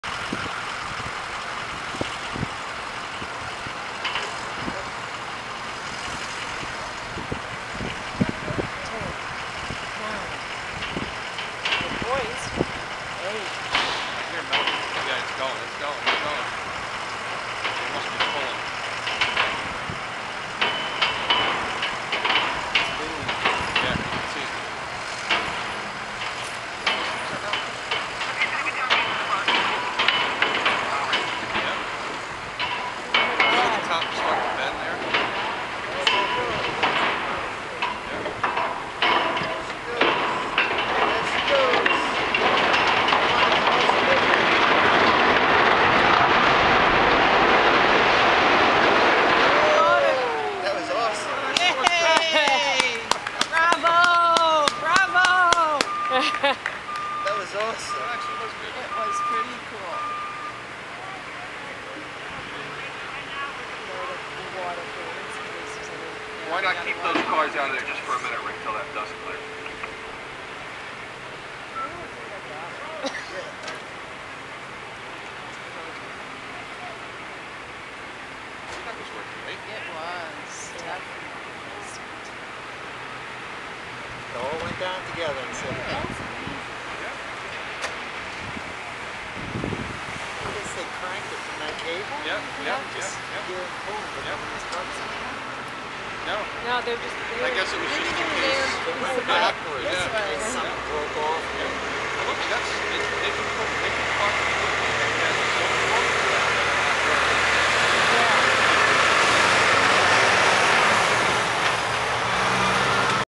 Demolition-Anaconda.avi
Demolition of former Anaconda factory in the village of Hastings on Hudson NY